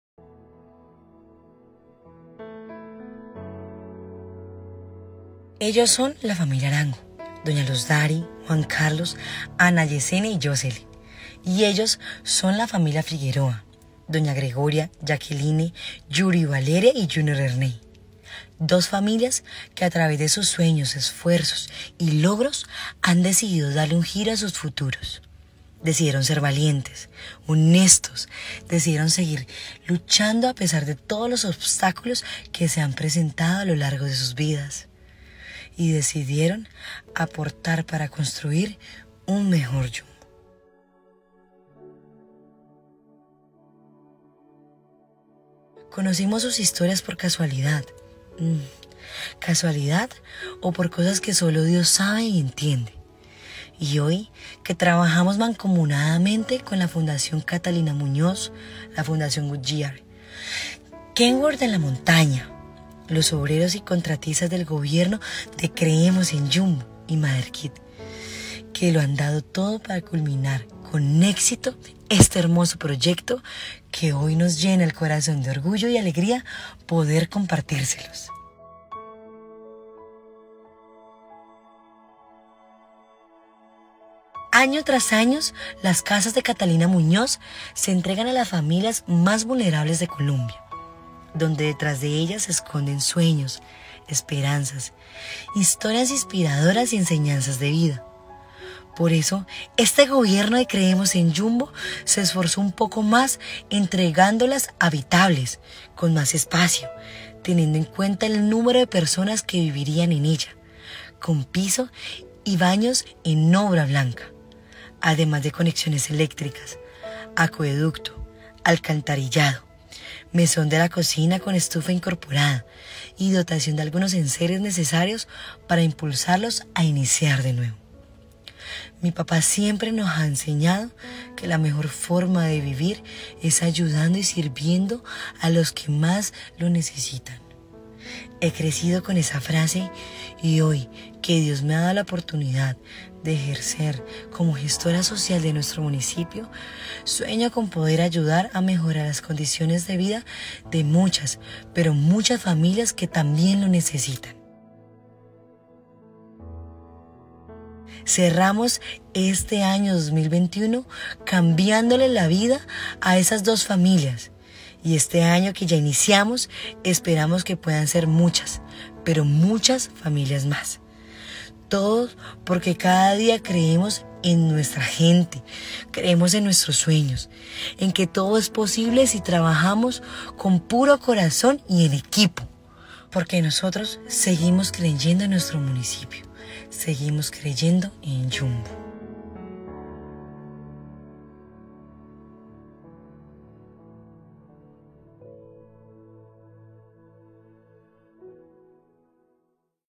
Una Casa Nueva Para Las Familias Arango y Figueroa
Hoy les mostramos con orgullo el resultado del trabajo mancomunado entre la Fundación Catalina Muñoz, la Fundación Goodyear, kenworth de la Montaña, los obreros y contratistas del Gobierno 'Creemos en Yumbo' y MaderKit que lo han dado todo para culminar con éxito este hermoso proyecto.
Cerramos el año 2021 cambiándole la vida a las familias Arango y Figueroa, dotándolas con una casa nueva, habitable y algunos enceres necesarios para que se impulsen y mejoren su calidad de vida, porque han demostrado que a pesar de las adversidades, son pujantes, familias soñadoras y llenas de valores, y este año que ya iniciamos, esperamos que puedan ser muchas familias más, porque creemos en que todo es posible si trabajamos con puro corazón y en equipo.